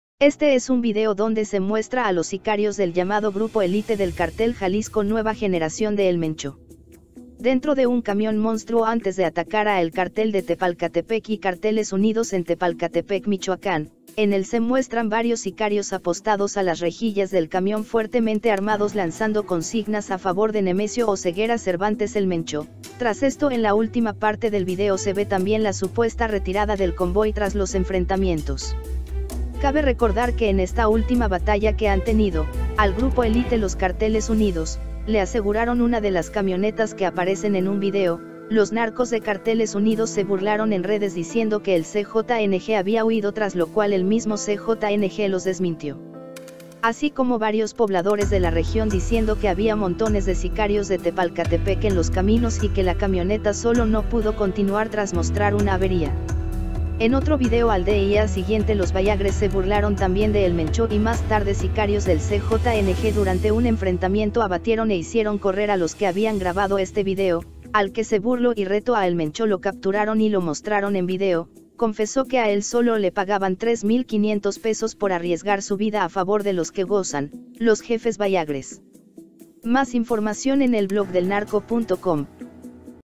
Sicarios del CJNG dentro de camión monstruo antes de atacar a Carteles Unidos en Michoacán
Este es un video donde se muestra a los Sicarios del llamado Grupo Elite del Cartel Jalisco Nueva Generación de El Mencho.

VIDEO Sicarios del CJNG dentro de camión monstruo antes de atacar a Carteles Unidos en Michoacán

Dentro de un camión monstruo antes de atacar a el Cartel de Tepalcatepec y Carteles Unidos en Tepalcatepec Michoacán, en el se muestran varios sicarios apostados a las rejillas del camión fuertemente armados lanzando consignas a favor de Nemesio Oceguera Cervantes El Mencho, tras esto en la última parte del video se ve también la supuesta retirada del convoy tras los enfrentamientos.